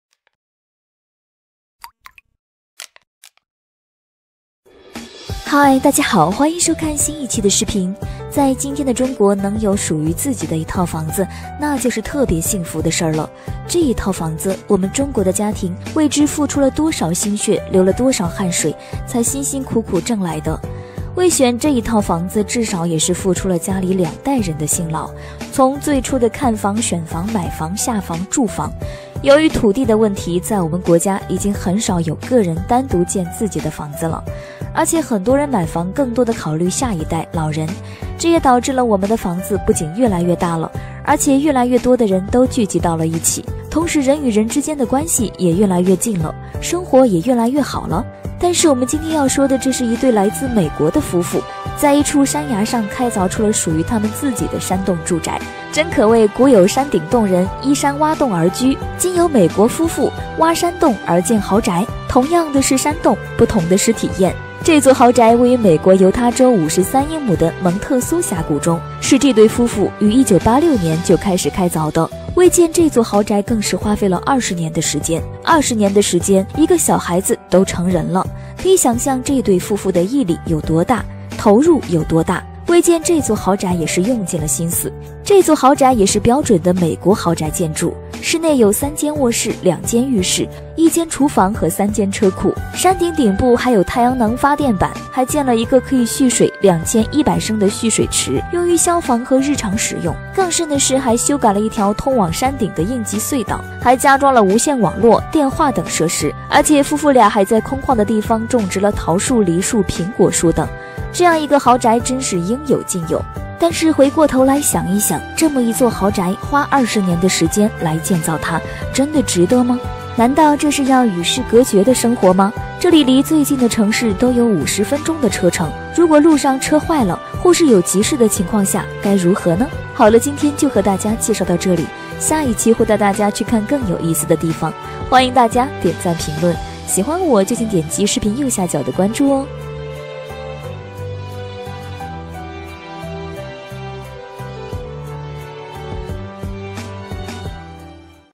美国夫妻花费20年的心血，在山里挖出一栋200平米的豪宅！
美国夫妇挖山洞而建豪宅, 这座豪宅位于美国犹他州53英亩的蒙特苏峡谷中, 是这对夫妇于1986就开始开凿的, 为建这座豪宅更是花费了20年的时间, 20年的时间, 一个小孩子都成人了, 可以想像这对夫妇的毅力有多大!